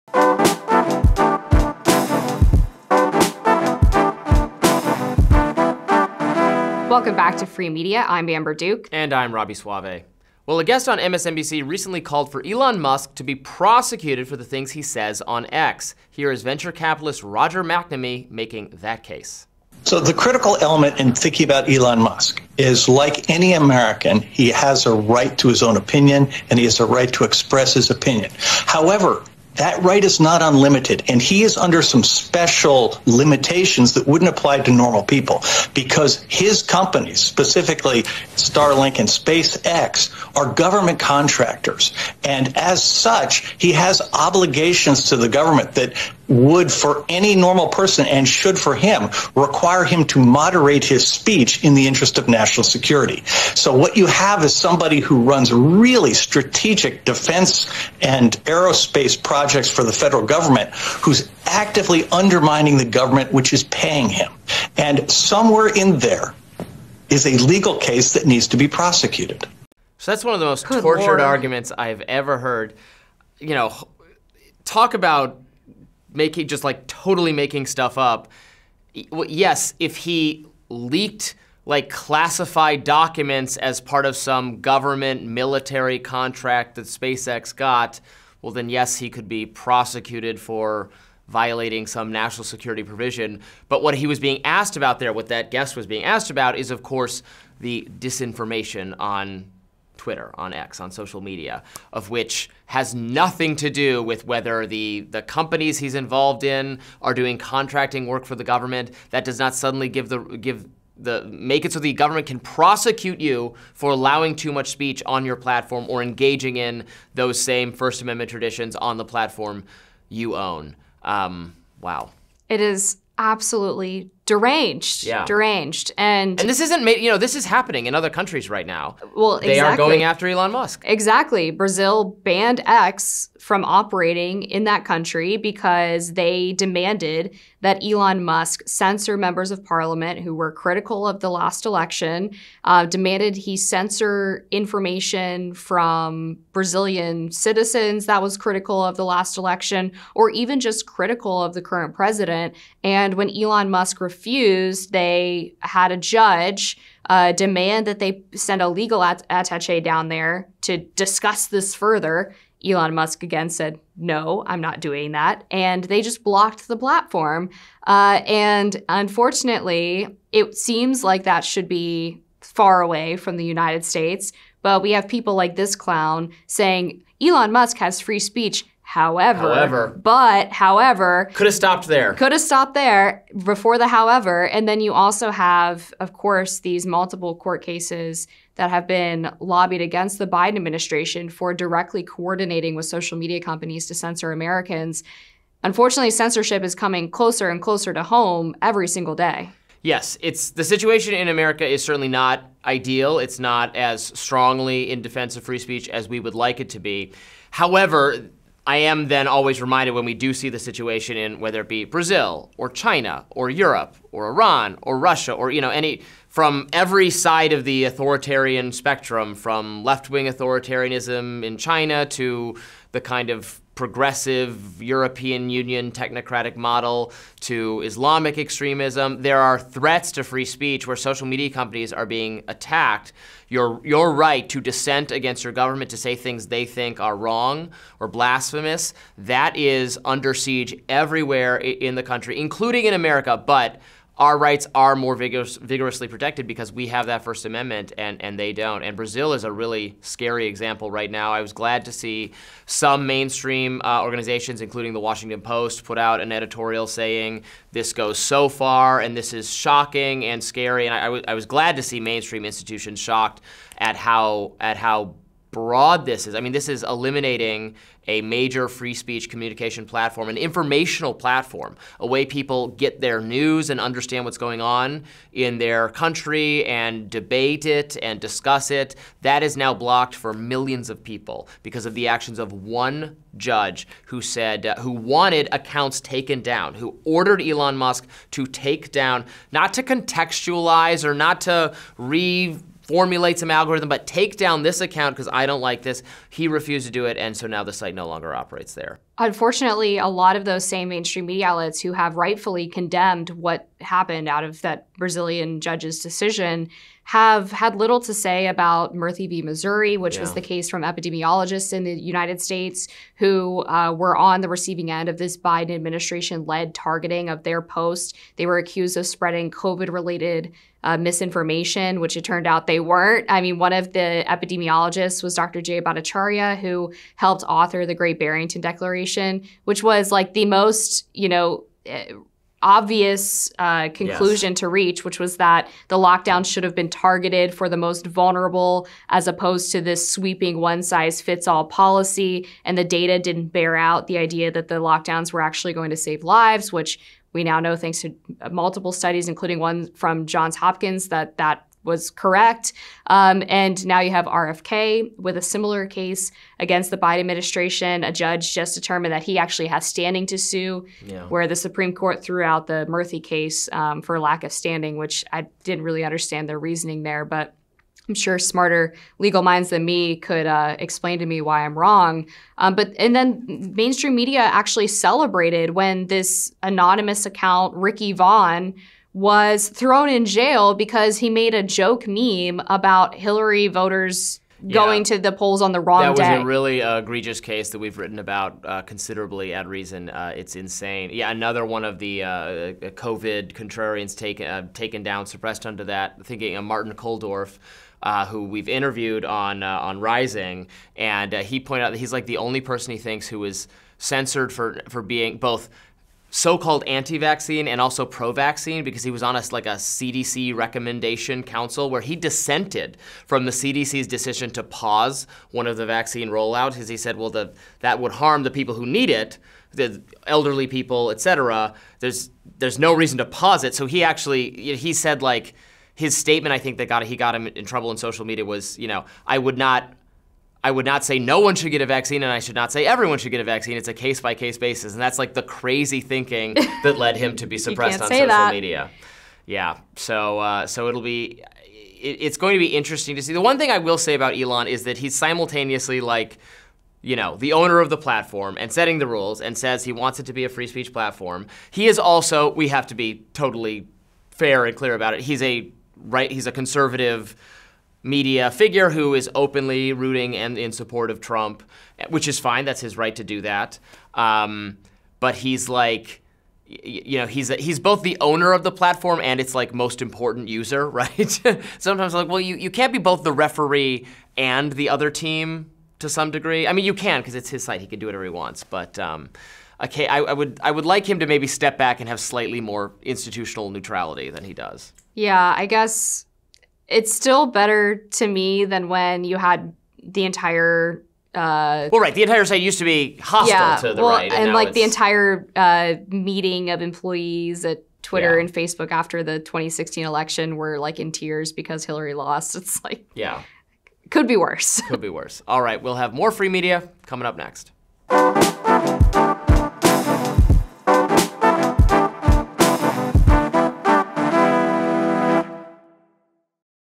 WATCH: MSNBC guest wants to PROSECUTE Elon Musk for speech
Robby Soave and Amber Duke discuss how Elon Musk has the same free speech protections as everyone else in the United States.

Photo credit: Tom Williams/CQ Roll Call/Newscom/MSNBC